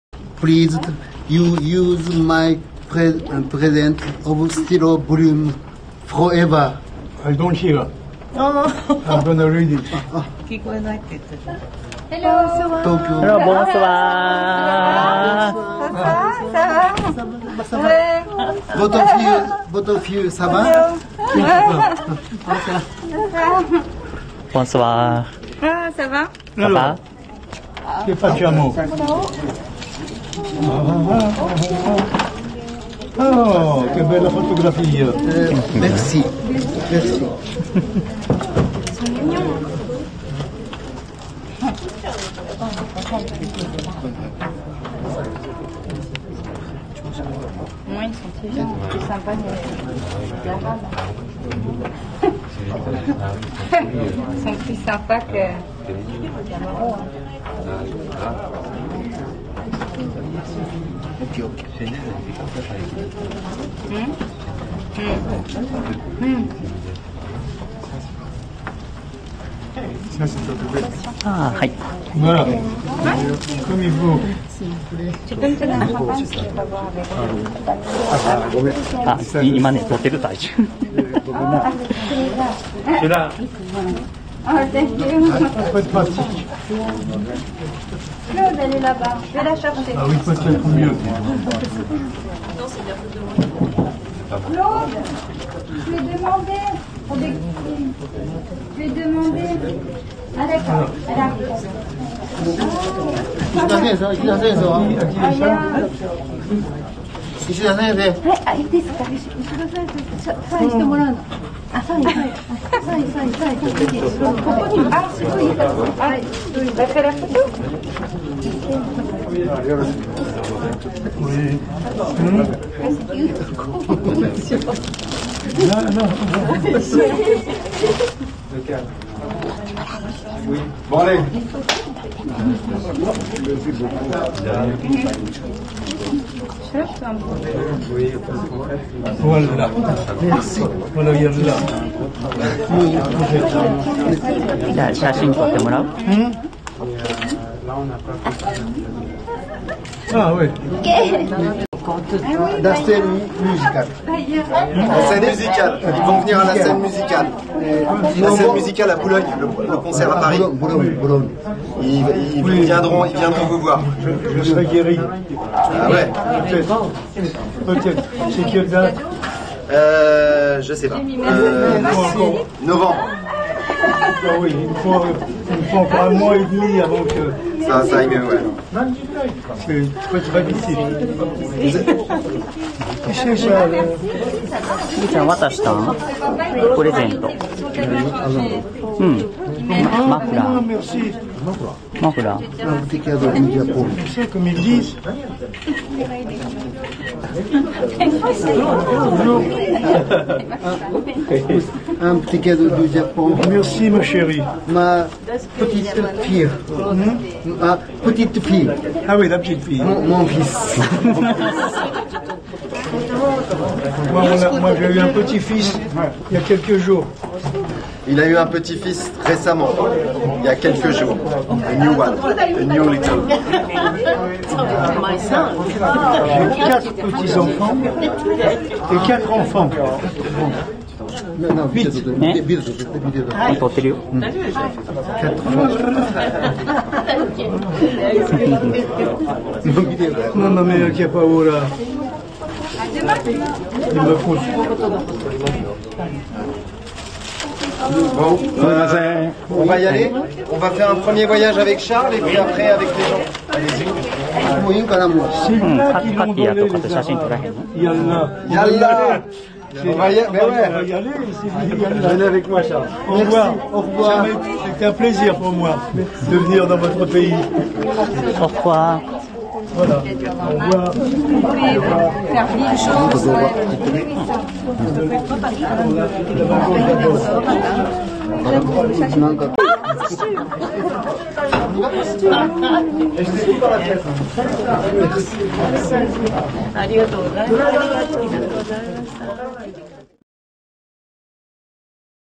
Charles Aznavour backstage after his last concert in Osaka on September 19, 2018
Après chacun de ses concerts, même épuisé et âgé de 94 ans, Charles Aznavour consacrait de longs moments pour rencontrer ses admirateurs. L’œil toujours coquin et d’humeur à plaisanter, le voici lors de sa toute dernière tournée triomphale au Japon. Encore convalescent de la terrible chute qui lui a causé la plus longue pause de sa carrière, il a réussi le tour de force de remonter sur scène, alors que beaucoup n’y croyaient plus.
Après un premier concert à Tokyo le 17 septembre, nous le retrouvons ici à l’issue de son ultime concert, le mercredi 19 septembre 2018 à Osaka.
Séquence très émouvante où il nous donne rendez-vous pour son prochain concert parisien le 8 novembre en promettant d’être en meilleure forme et totalement rétabli.
Il annonce aussi avec une joie immense la naissance de Missak, son dernier petit fils.
Merci à Yoshizo Okamoto pour cette vidéo émouvante.
Charles Aznavour devait nous quitter le lundi 1er octobre 2018.
PS : Mention spéciale pour le “traducteur” ;)